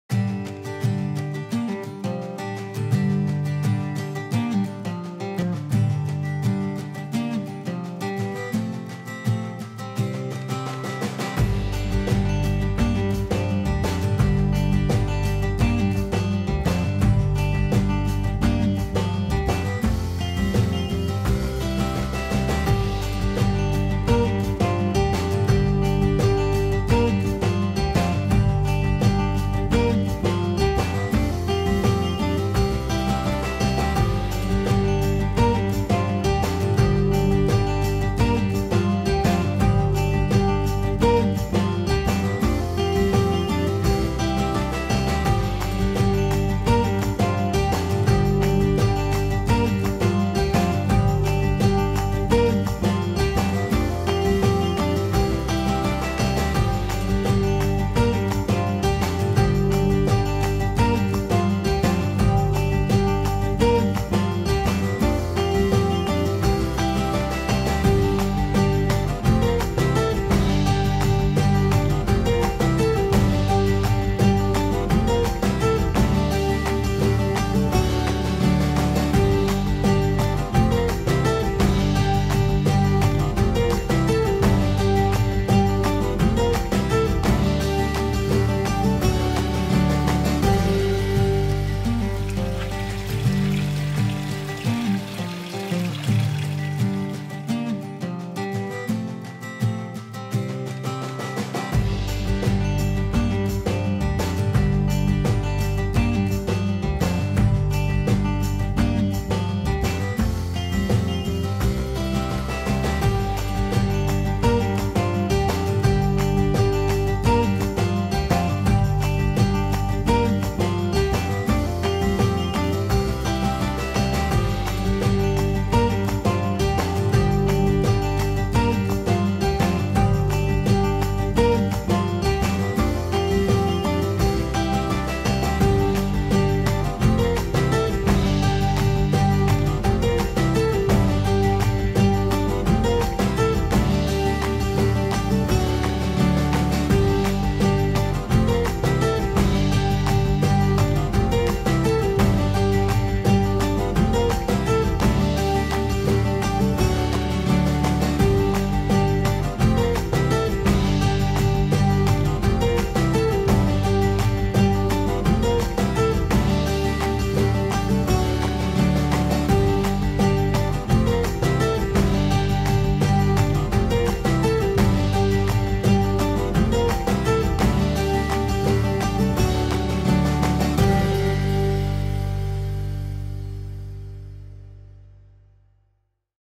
Beautiful Ranch Linting -1 of a kind
32 ac. working ranch with multiple pastures perfect for cattle and horses. Hay barns and storage throughout. Here is a video of a listing in Sanger Ca. Listing agent Mid Valley Realty in Coalinga Ca.

RADSTUDIOSVIDEOGRAPHY.COM